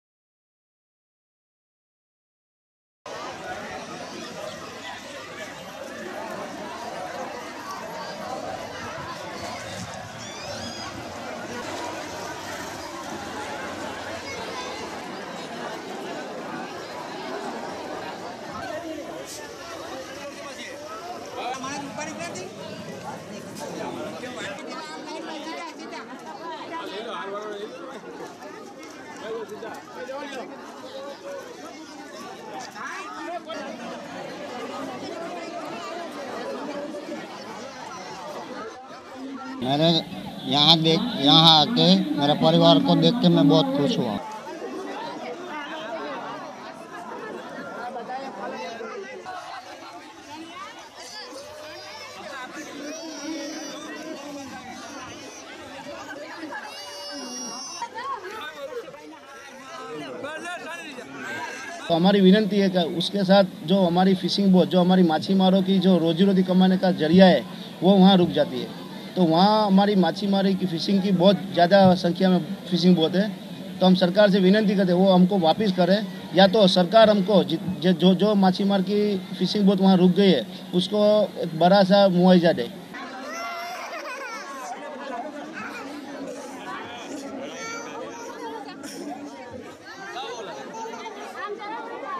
Fishermen return home in west India after release from Pakistan
Indian fishermen, released from Pakistani prisons, reach their homes in western state of Gujarat and express jubilance on being united with family.